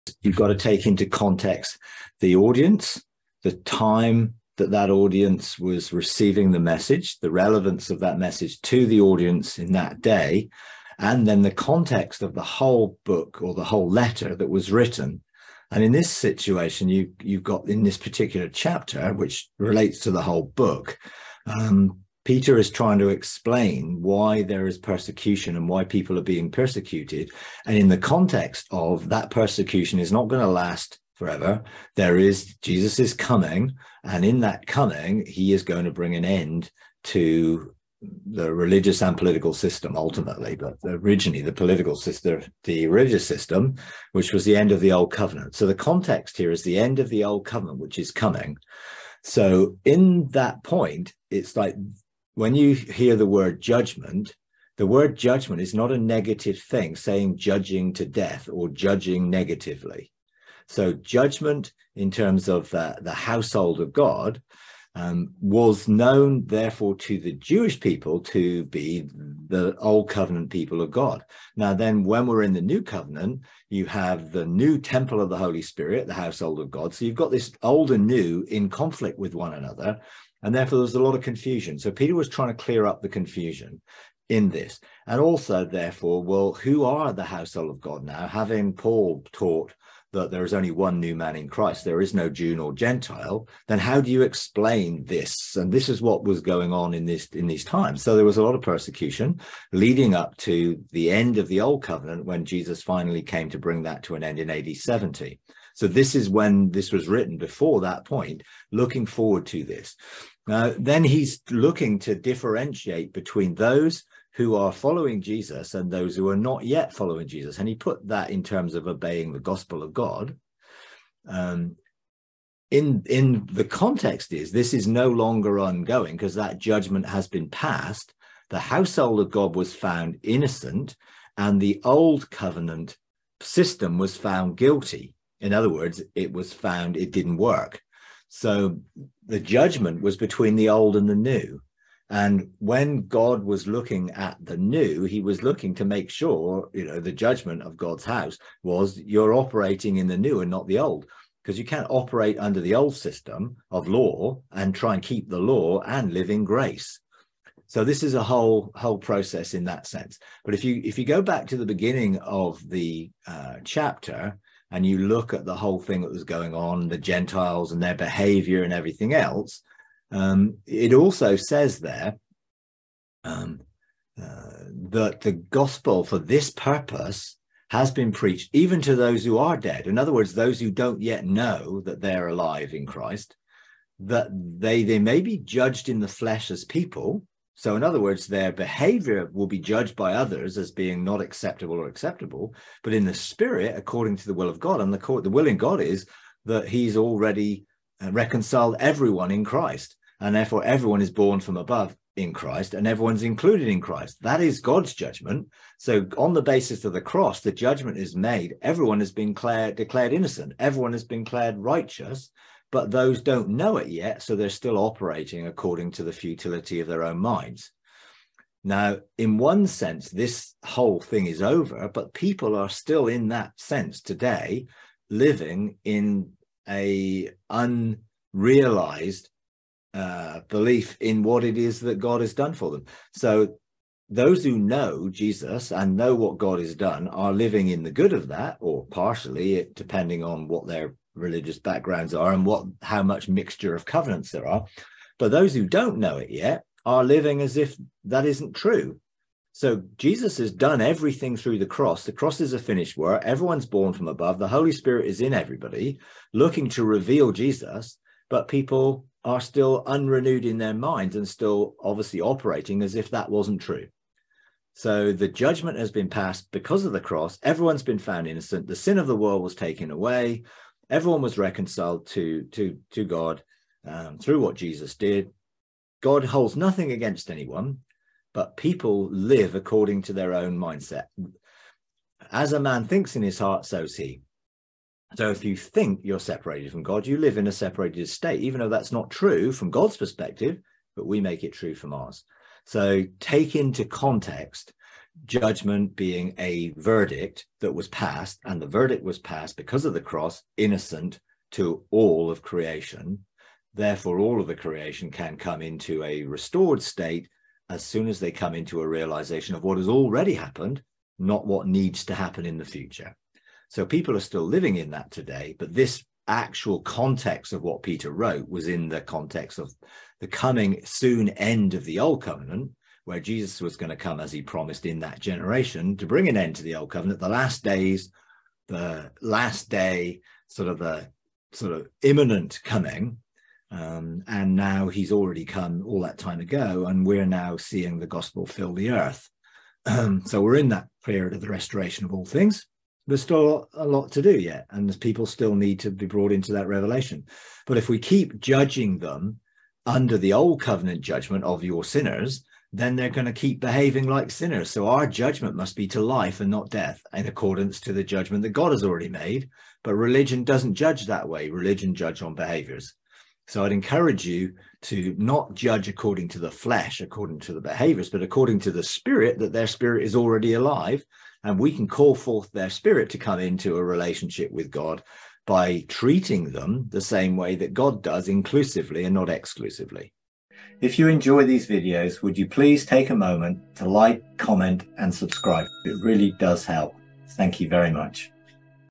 Understanding Persecution and the End of the Old Covenant | Exploring Biblical Context
Mike delves into the intricate details of biblical context, exploring key themes such as the audience and time period, the relevance of the message, and the impending end of the Old Covenant. He discusses Peter's explanation of persecution, the differentiation between followers and non-followers, and the importance of obedience to the Gospel of God. Furthermore, Mike touches on the concepts of judgement and the household of God, as well as the behaviour of Gentiles and the purpose of the Gospel.

1. Introduction to Biblical Context - 00:00
2. Understanding the Audience and Time - 00:45
3. Exploring the Relevance of the Message - 01:30
4. Peter's Explanation of Persecution - 02:15
5. The Impending End of the Old Covenant - 03:00
6. Differentiation Between Followers and Non-Followers - 03:45
7. Obedience to the Gospel of God - 04:30
8. Judgement and the Household of God - 05:15
9. Gentiles' Behaviour and the Purpose of the Gospel - 06:00